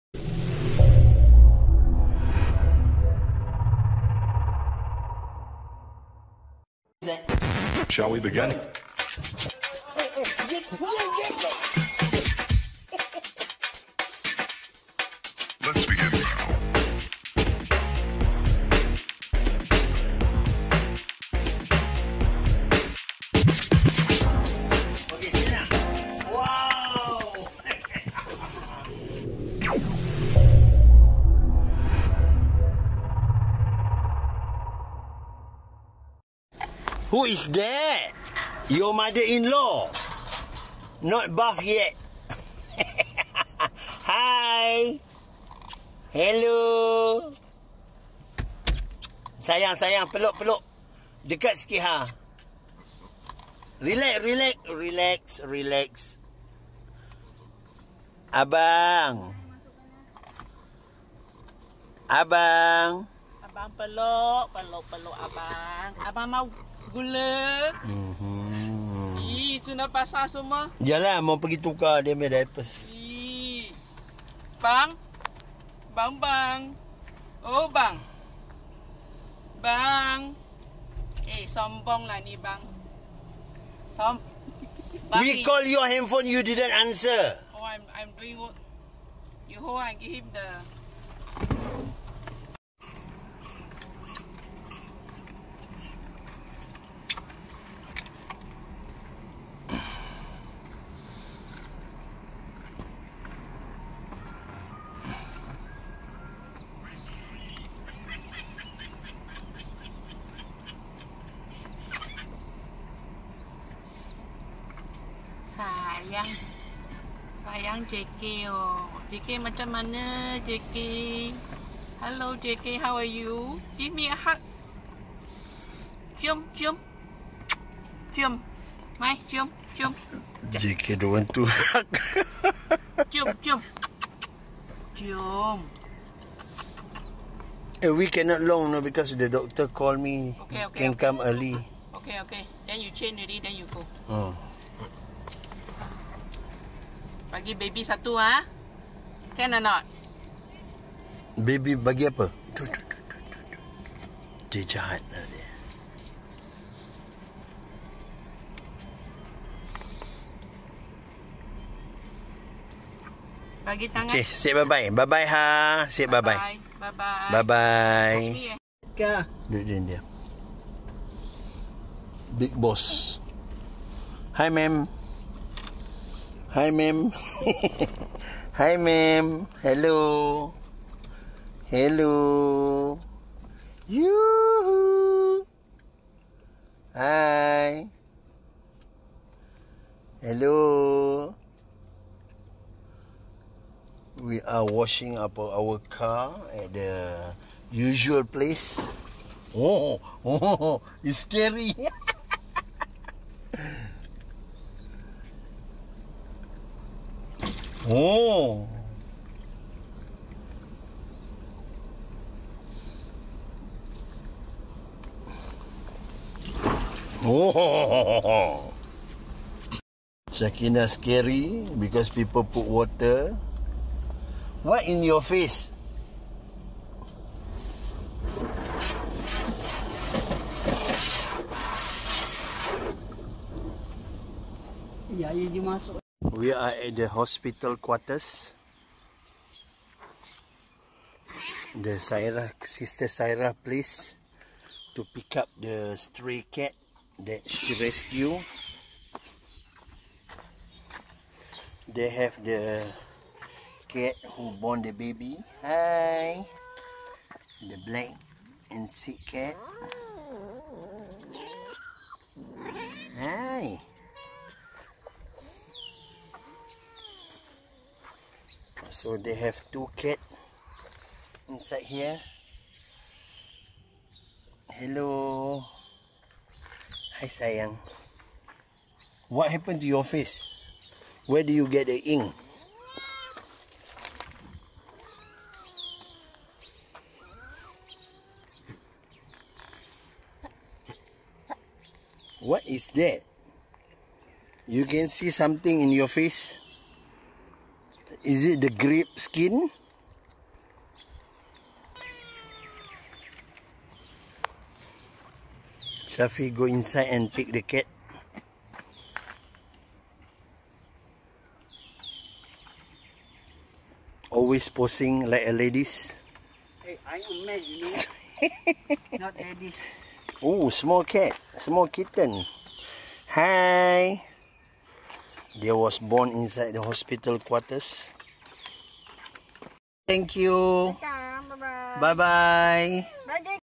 Visite Sister Yock Har and Rescue Stray Cats ( need rest Tonight )
Arrive at 8.00 pm highwaybjem.Heavy raining

For Thos Who are Interested to Support me in helping Animals, you can do below ya.